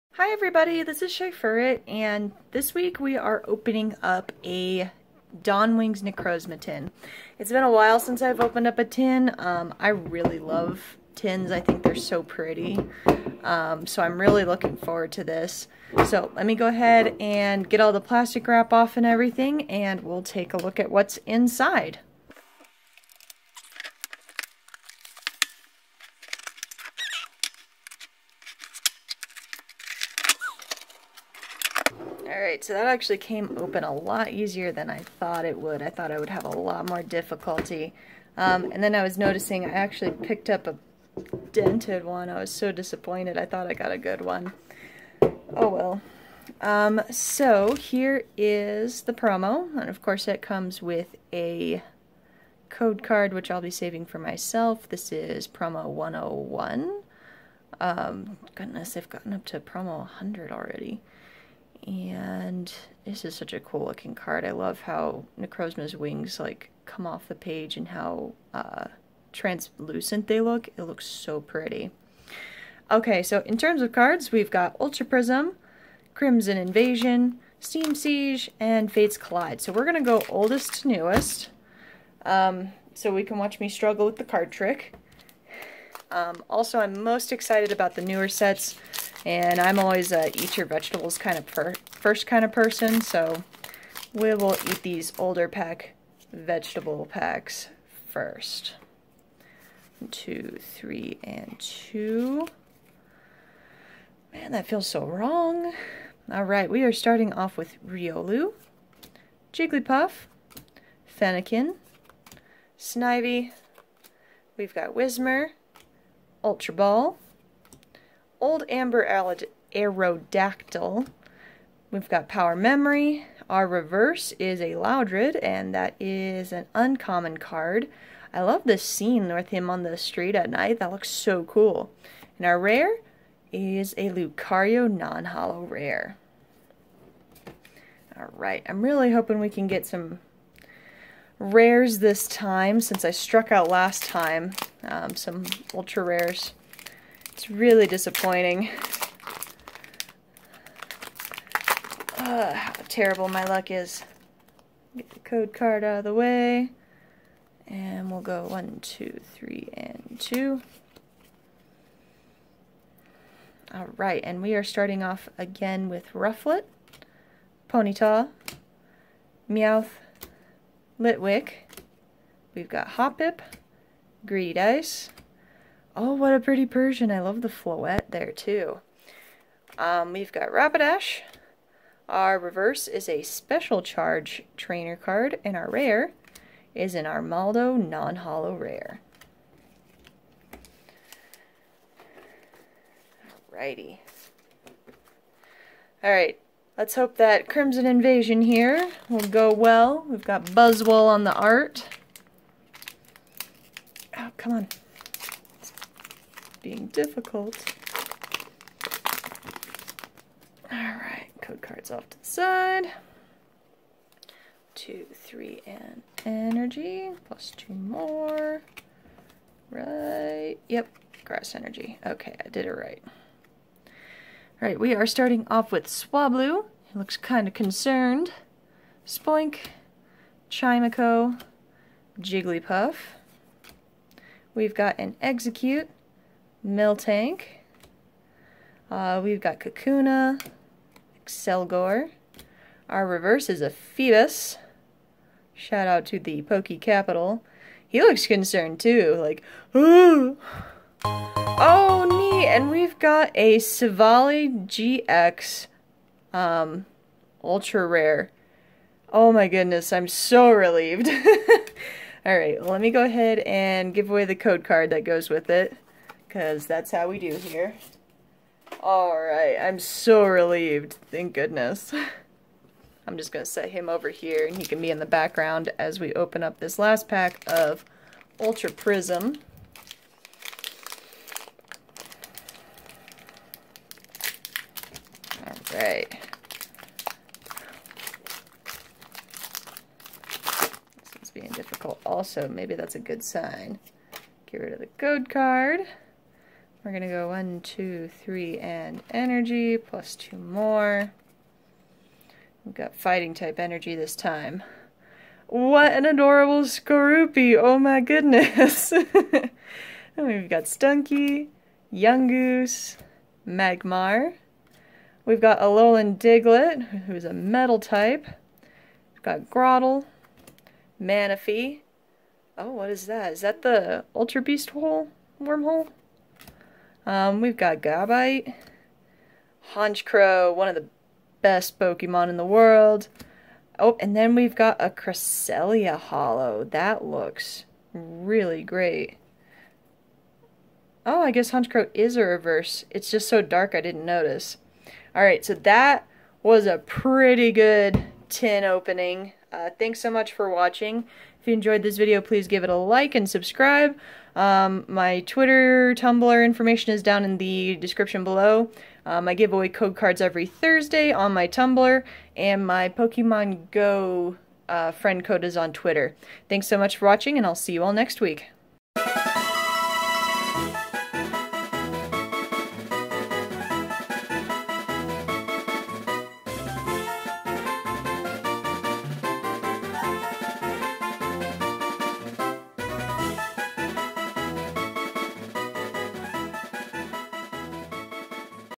Dawn Wings Necrozma Tin Opening
This week we're opening a Dawn Wings Necrozma tin, and it turns out very well.
Filmed on my Samsung Galaxy S7